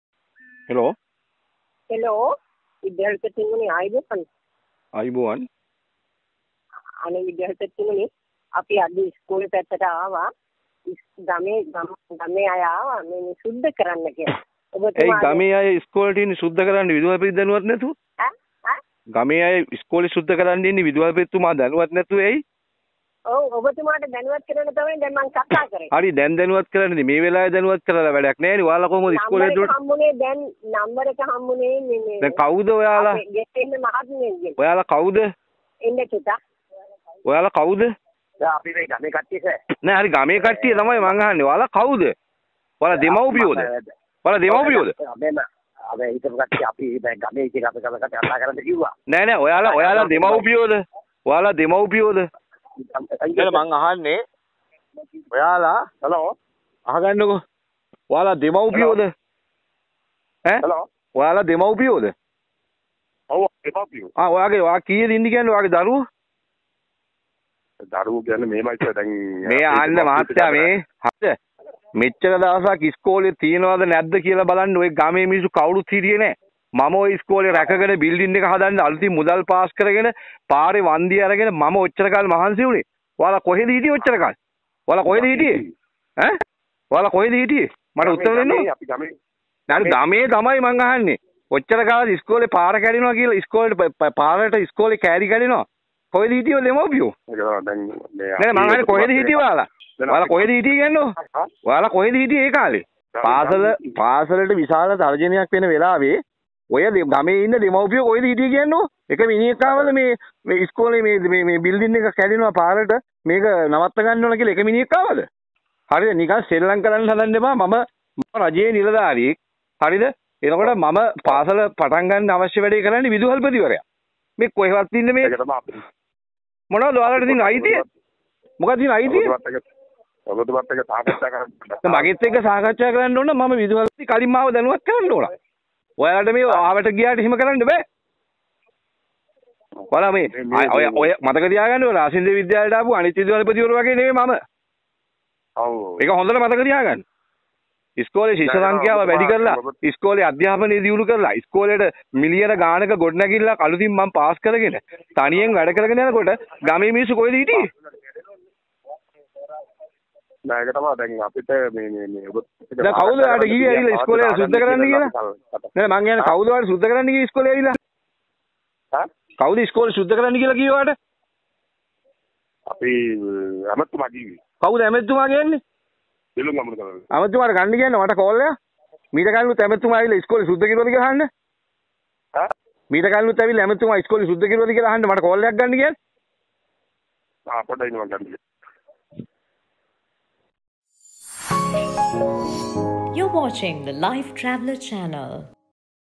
පාසල සුද්දකරන්න බලෙන් ආ ඇමති ගෝලයන්ට විදුහල්පති හොදට කියා දෙයි
පාසල සුද්දකරන්න බලෙන් ආ ඇමති ගෝලයන්ට විදුහල්පතිගෙන් වාග් ප්‍රහාරයක්..